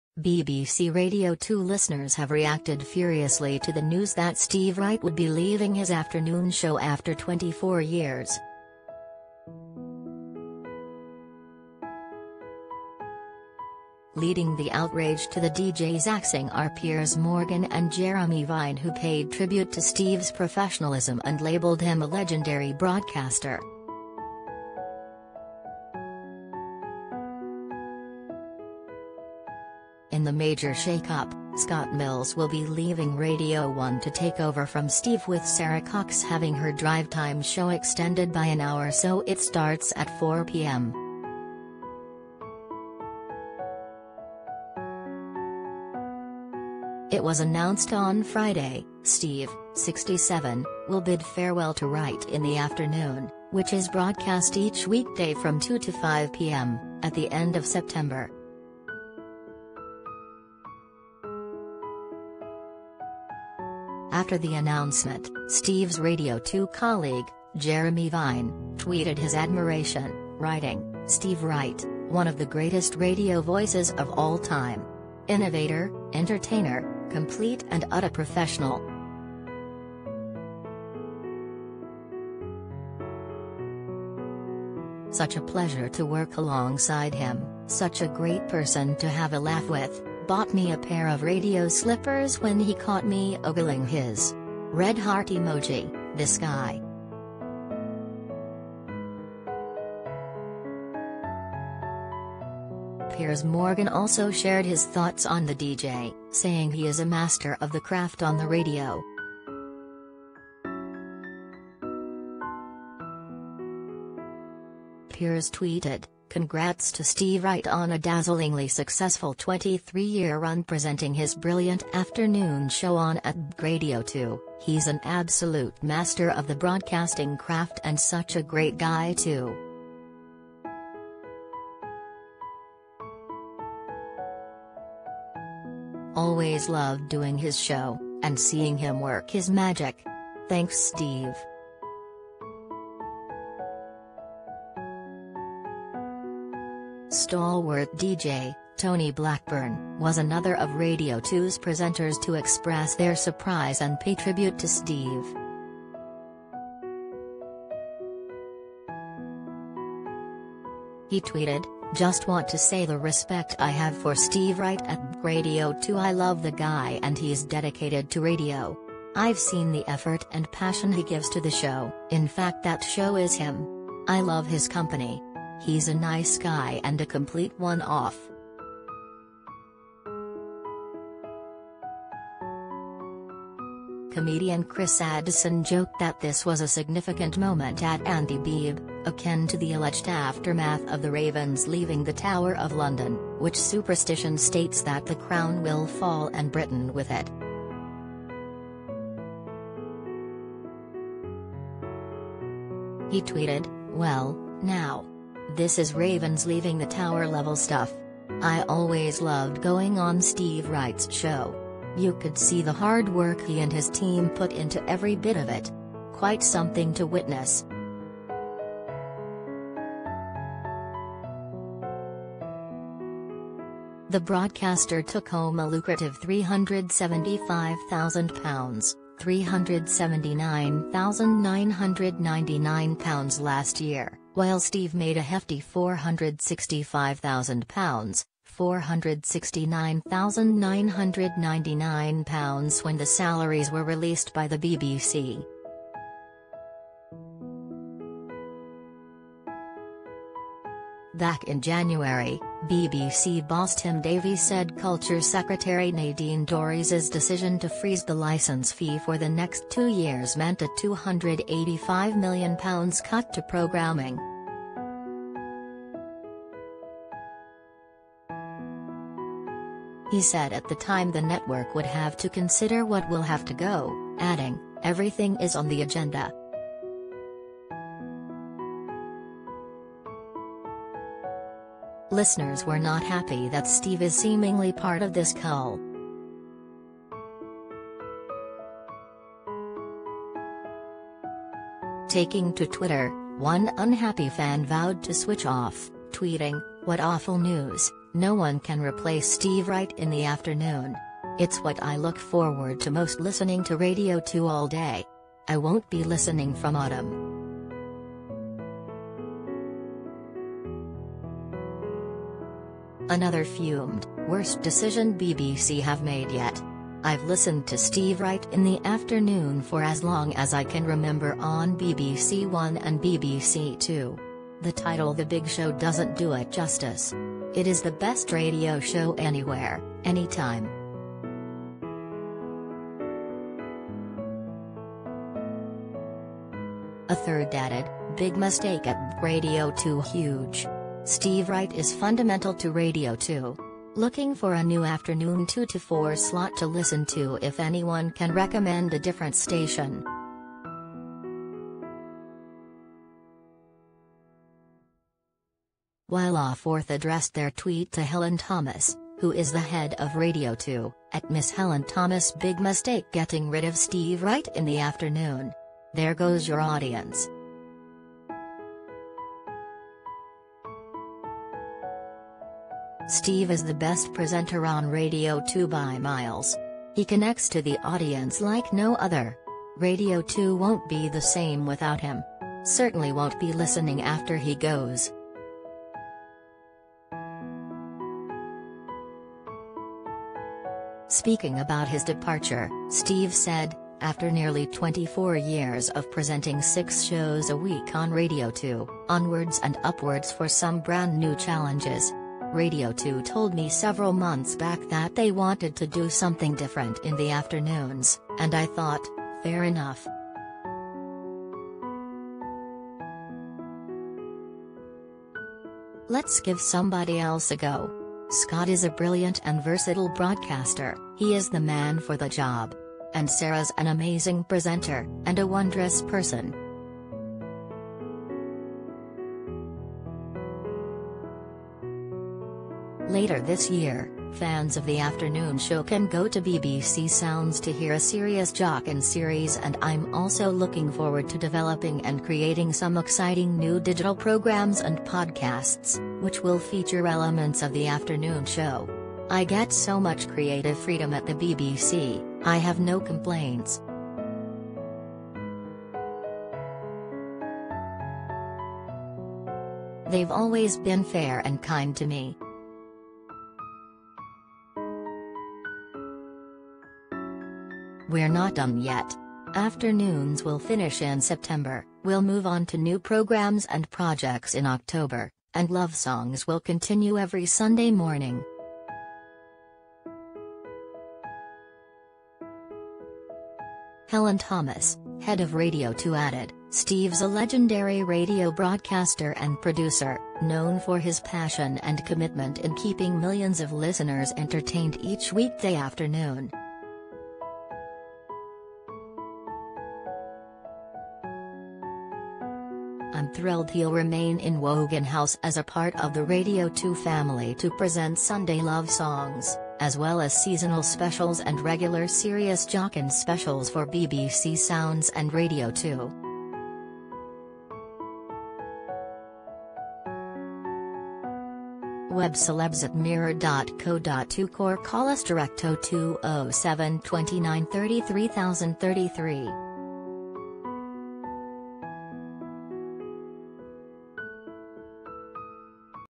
Piers Morgan and Jeremy Vine speak out after Steve Wright's shock radio axing
BBC Radio 2 listeners have reacted furiously to the news that Steve Wright would be leaving his afternoon show after 24 years.
Leading the outrage to the DJ's axing are Piers Morgan and Jeremy Vine who paid tribute to Steve's professionalism and labelled him a 'legendary' broadcaster.
In the major shake-up, Scott Mills will be leaving Radio 1 to take over from Steve with Sara Cox having her drivetime show extended by an hour so it starts at 4pm.
It was announced on Friday, Steve, 67, will